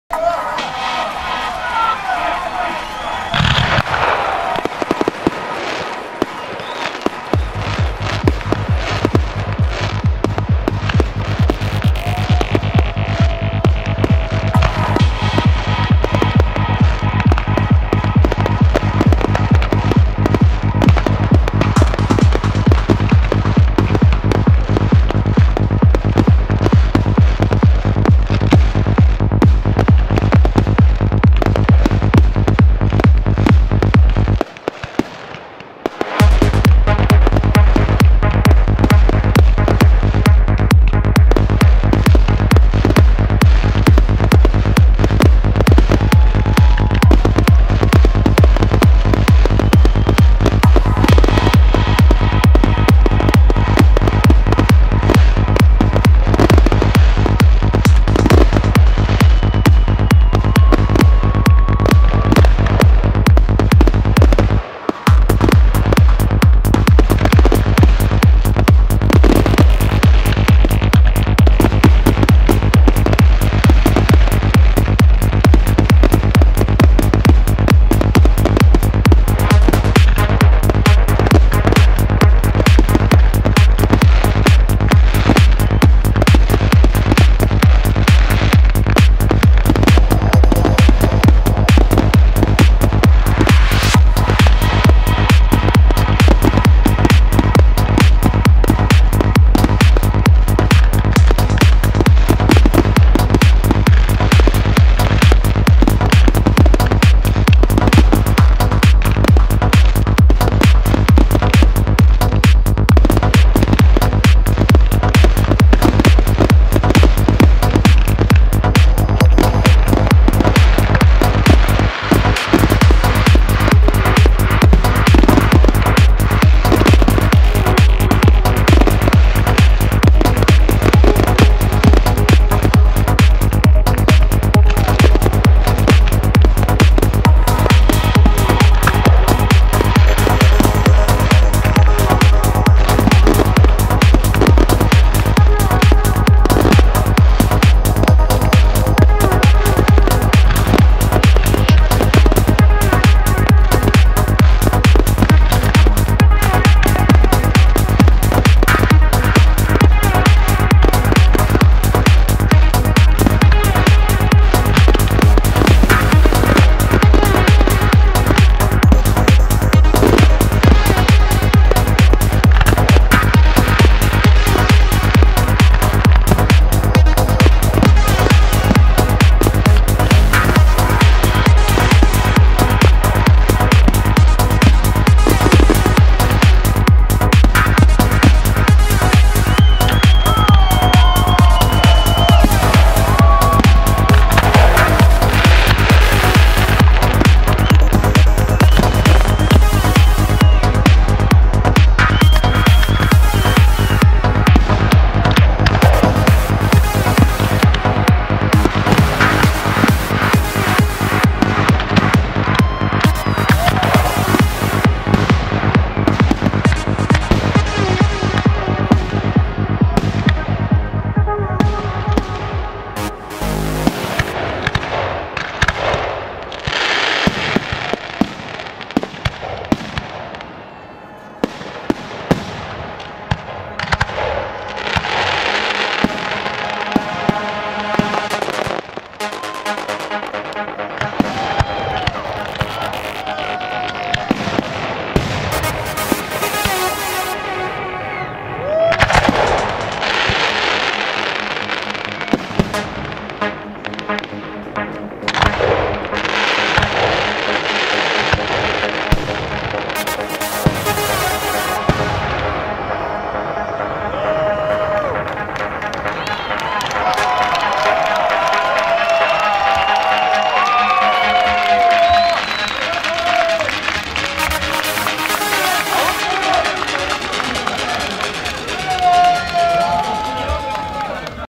Профессиональный салют на корпоративе Москва
Отличный салют на корпоративе одной из успешных московских кампаний. Правильный салют - только для состоятельных парней !